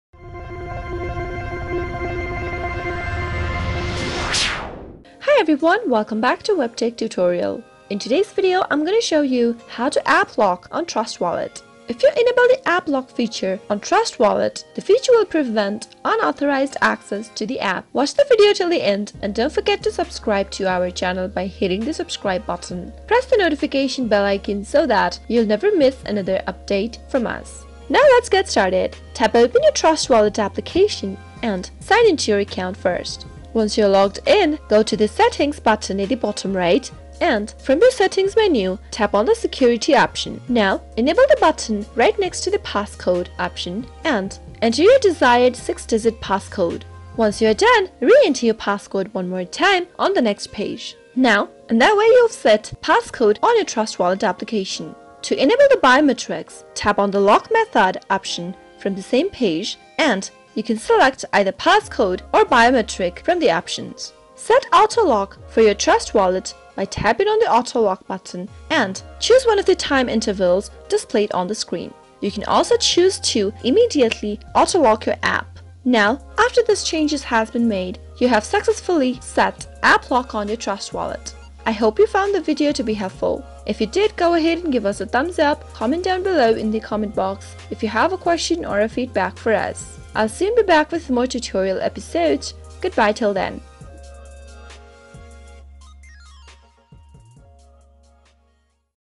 How to App Lock On Trust Wallet 2021?
KEEP YOUR CRYPTO CURRENCY SAFE In the following video, I present you with how to app lock a trust wallet. Cryptocurrencies are getting more prolific but unfortunately, so are scammers and their schemes. While blockchain networks are notoriously difficult to hack, cybercriminals often find ways to manipulate users to gain access to their funds. In this video, you will learn how to tighten your wallet security as a Trust Wallet user.

The following are some of the videos, that you need to follow to know how to app lock a trust wallet.
Step 1:First tap open the trust wallet app and sign in if you haven't.
Step 2: Once you log in to your account, go on to the setting button on the bottom right corner.
Step 3: Now, go and tap on the security option from the expanded menu.
Step 4: After that, enable the passcode, you've to type 6 digit passcode.
Step 5: Once you're done, re-enter the passcode on the next page.
Step 6: You can also tap on the auto-lock option and change the time of the passcode.

This is how you can lock on trust wallt. I hope this video was helpful to you.

0:00 Intro
0:29 How to app lock on trust wallet.
1:21 Outro